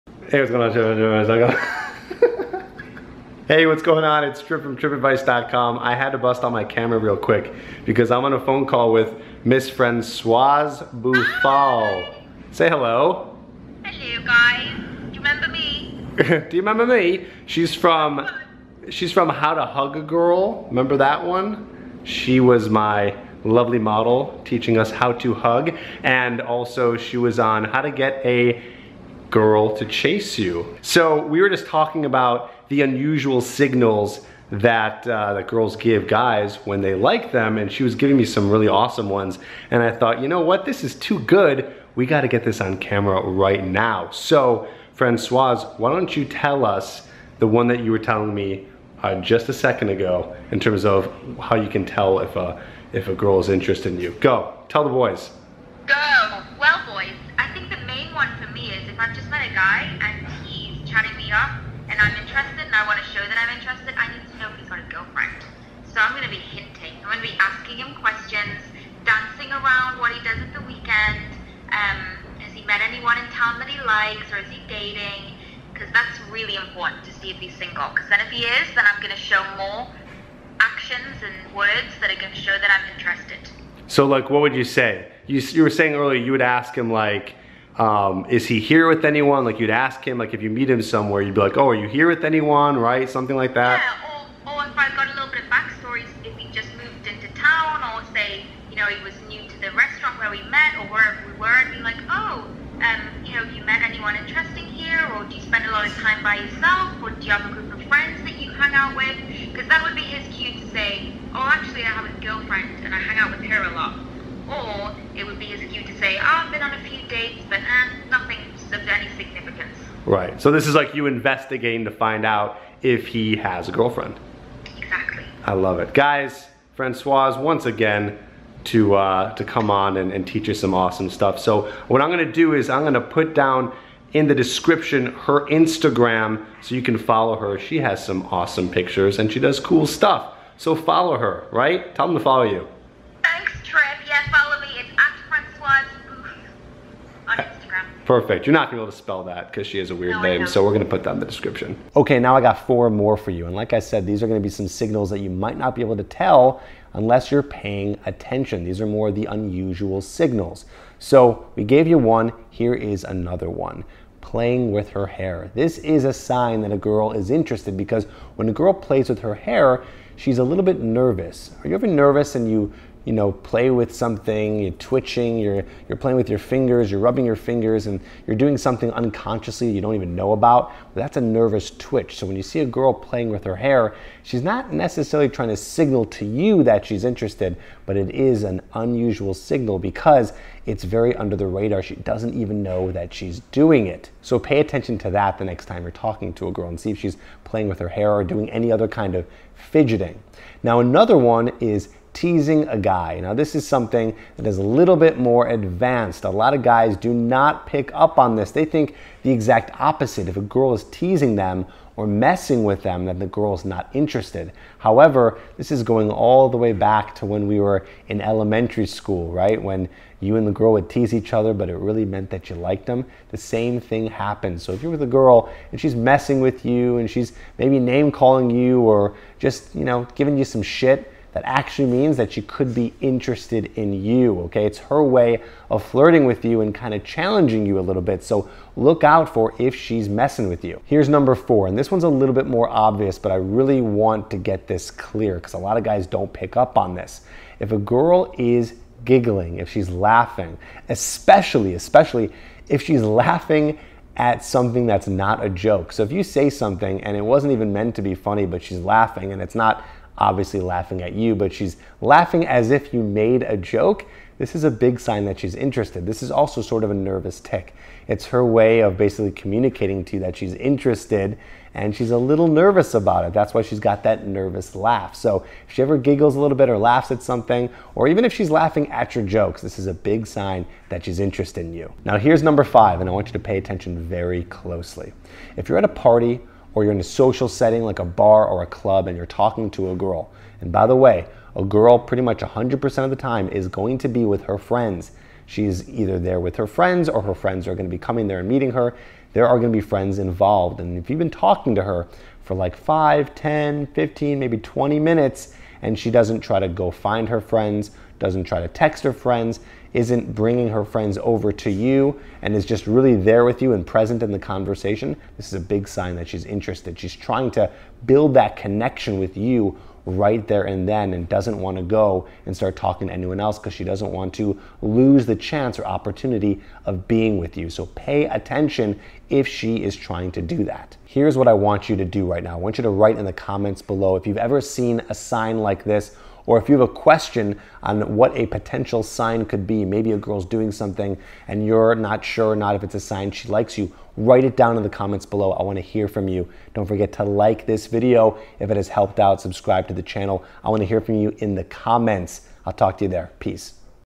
5 Unusual Signals Women Give Men They Like
You'll learn the 5 unusual signals women give when they like a man. Some of these you might have heard of before and others you may be hearing for the first time.